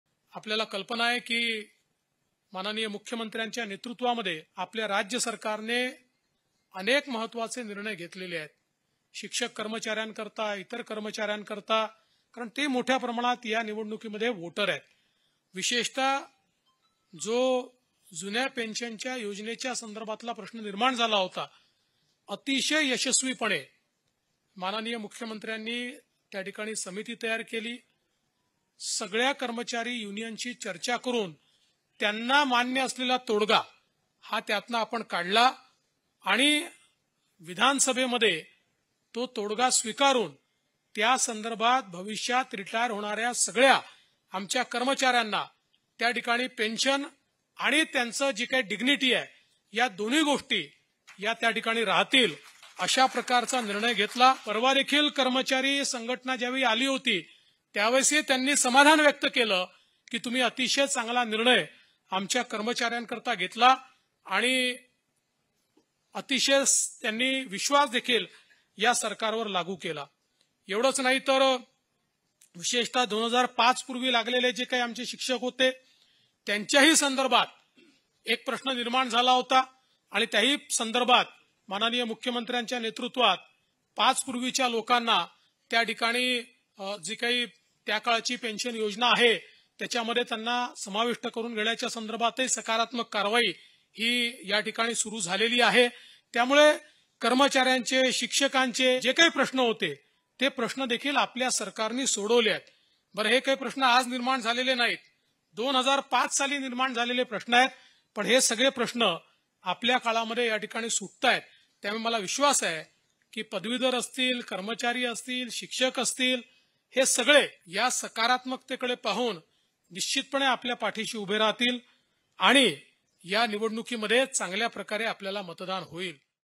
महायुती सरकारने घेतला 'या' कर्मचाऱ्यांच्या हिताचा निर्णय | ठाणे | DCM Devendra Fadnavis
महायुती सरकारने घेतला 'या' कर्मचाऱ्यांच्या हिताचा निर्णय | DCM Devendra Fadnavis
(महायुती विजय संकल्प मेळावा | ठाणे | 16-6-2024)